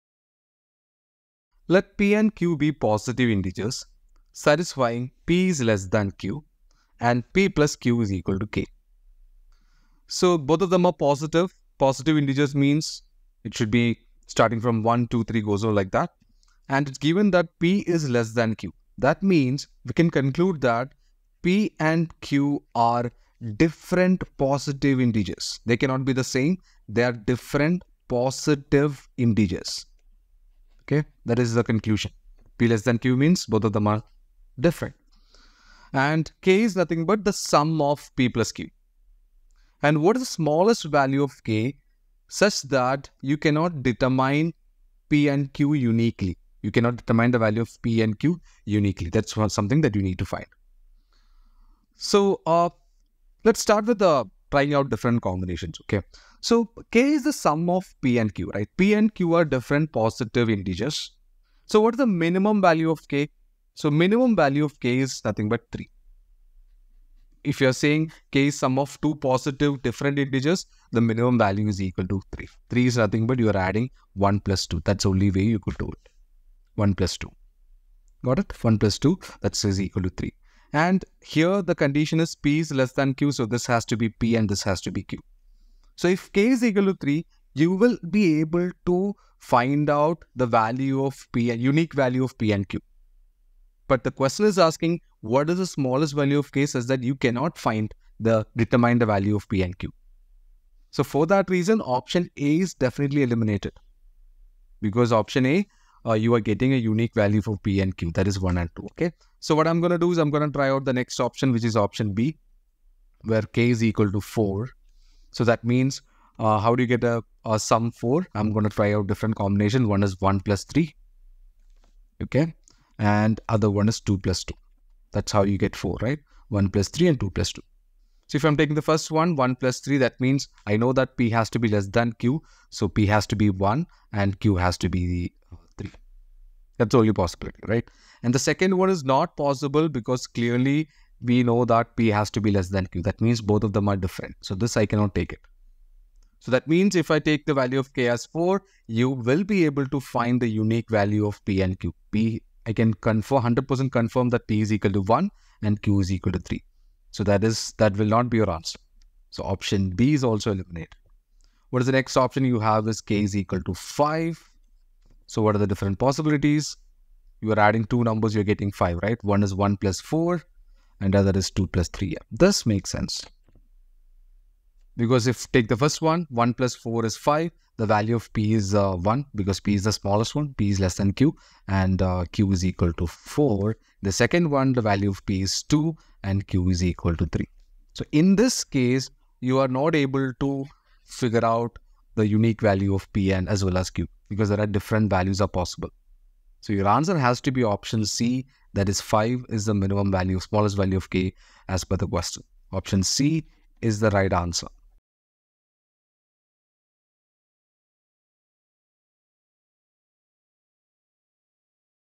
Let p and q be positive integers satisfying p... | CSAT 2024 | UPSC | ClearIAS
Let p and q be positive integers satisfying p ( q and p + q = k. What is the smallest value of k that does not determine p and q uniquely?

Let's look at the solution for this problem which was asked in the UPSC CSE Prelims CSAT paper of 2024

Join the ClearIAS CSAT Program to get instant access to already uploaded classes and automatic access to upcoming classes for just Rs.4999.